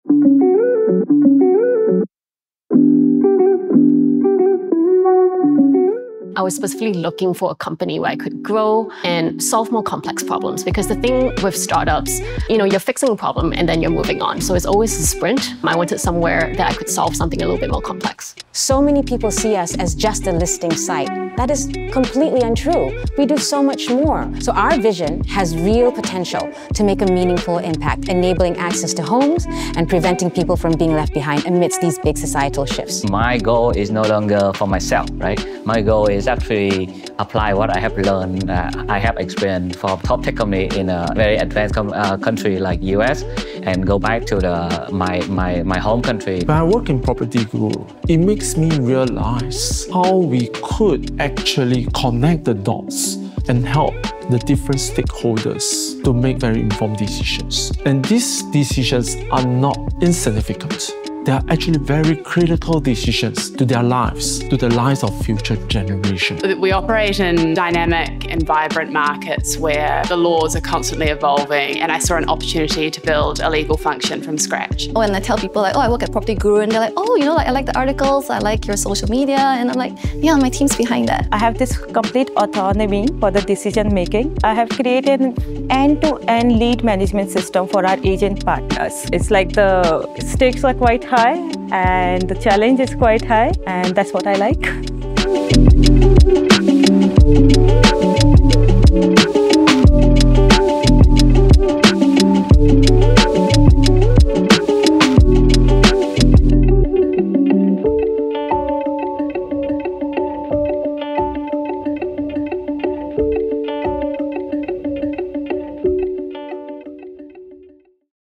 Be More, Be a Guru
Nhiều công ty khẳng định rằng công việc của họ không chỉ là một công việc, chúng tôi tin rằng chúng tôi còn mang đến nhiều hơn thế. Tại PropertyGuru Group, chúng tôi không chỉ giải quyết các vấn đề; chúng tôi kiến tạo tương lai. Dù là đột phá với nền tảng dữ liệu, hay là xây dựng những đội nhóm tuyệt vời, Gurus chúng tôi luôn đi đầu trong việc định hình bối cảnh PropTech.

Bạn hãy xem video để tìm hiểu những động lực đang thúc đẩy Gurus chúng tôi và chứng kiến tầm ảnh hưởng mà chúng tôi đang tạo ra khắp Đông Nam Á. Từ giải quyết các thách thức phức tạp, đến hỗ trợ các cộng đồng, mỗi Guru đều nỗ lực cống hiến chuyên môn cũng như niềm đam mê của mình nhằm tạo ra sự khác biệt.

Chúng tôi mang đến cho bạn những công cụ, niềm tin và sức mạnh để bạn có thể tạo ra sự thay đổi thực sự. Đây còn hơn cả một công việc -- Đây là cơ hội để bạn Be More, Be A Guru.

Hãy gia nhập cùng chúng tôi và tạo nên dấu ấn của bạn: